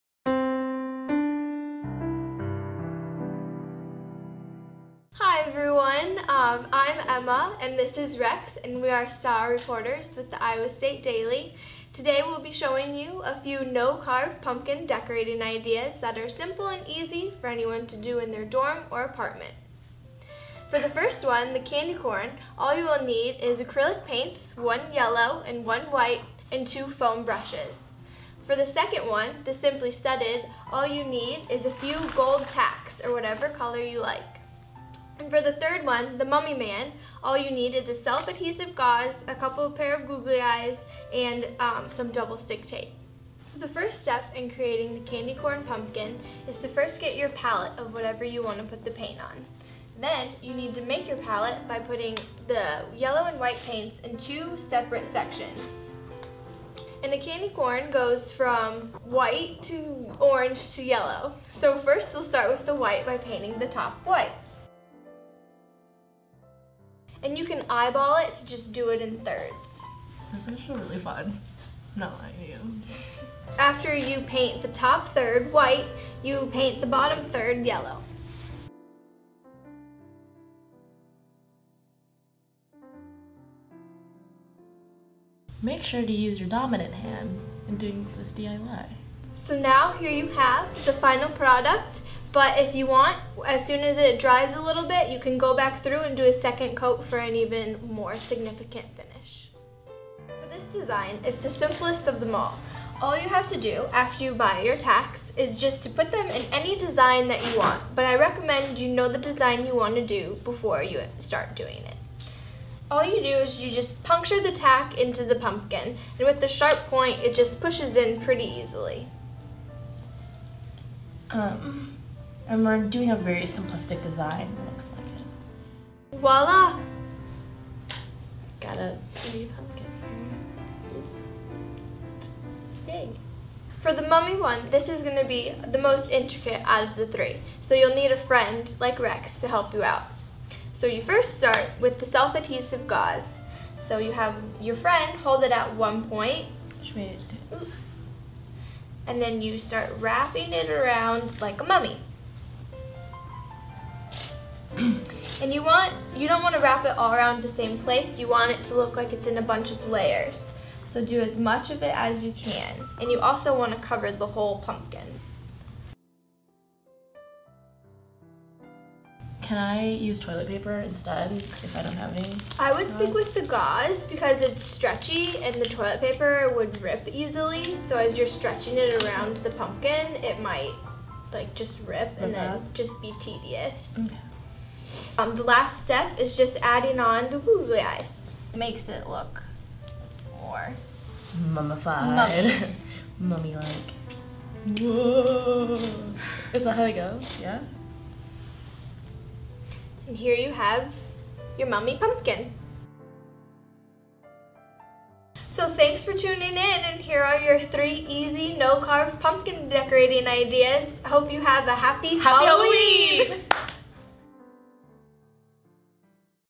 DIY: No-carve pumpkin decorating
Want a festive pumpkin for Halloween but don't want to go through the mess of carving?

Iowa State Daily style reporters Emma Kuntz and Rex Geronimo show you how to decorate pumpkins with simple household products. Instead of the mess of carving, these projects are ideal for students who live in residence halls and dorms or apartments.

in the Style section or the Multimedia tab.